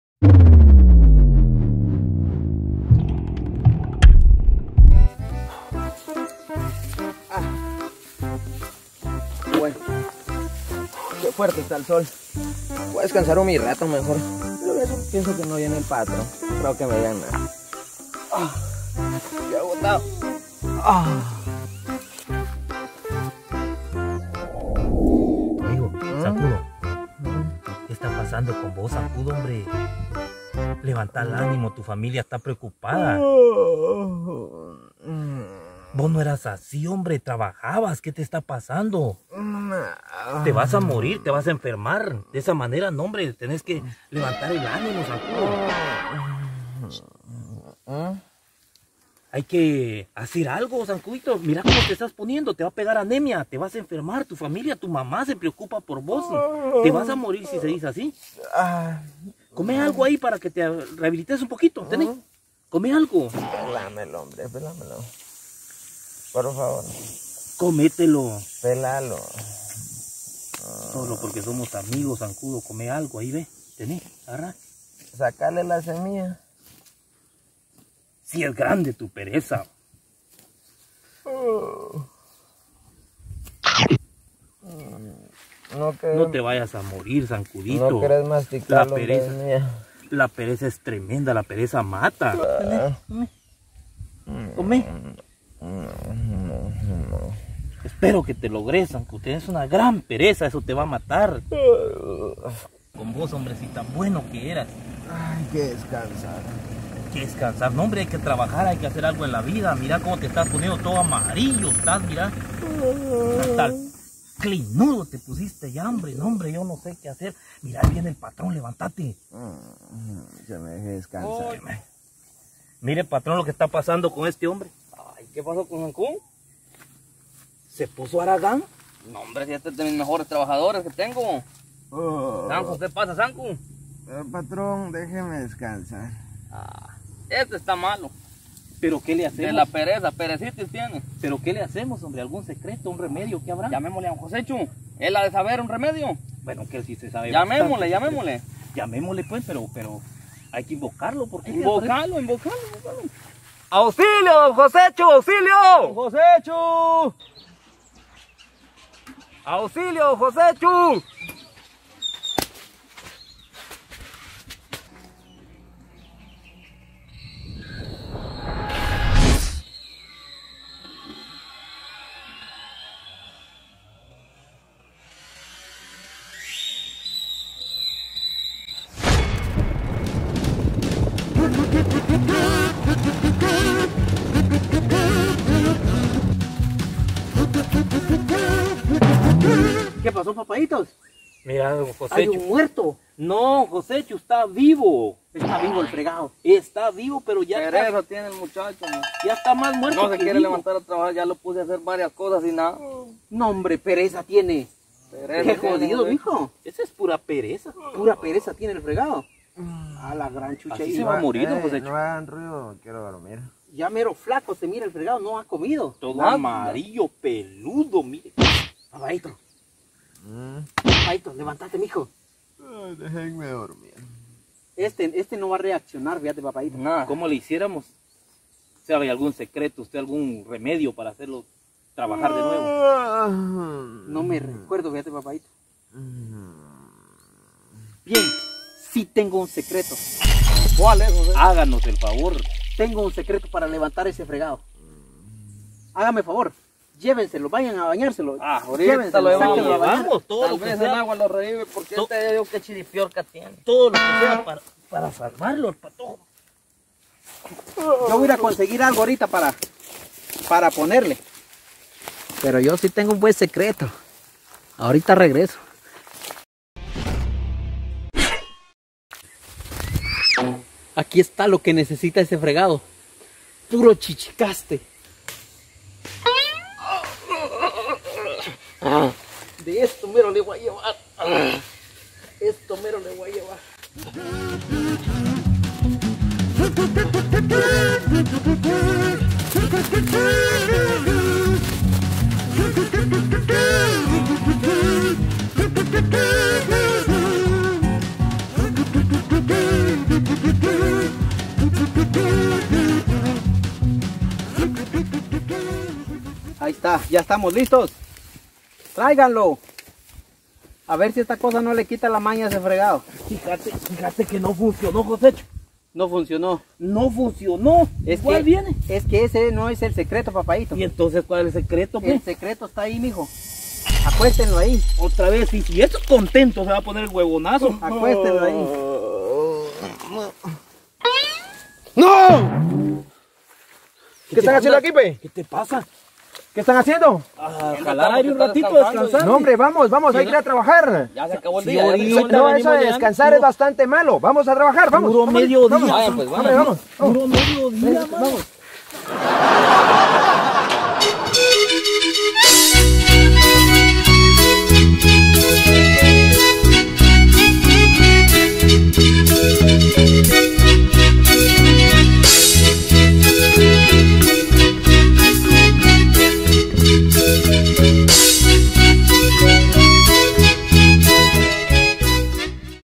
A puro Chichicaste curaremos a este Haragan jajaj
este muchacho parece estar muy enfermo pero existe un metodo para curarlo, Super Josechu si sabe jaja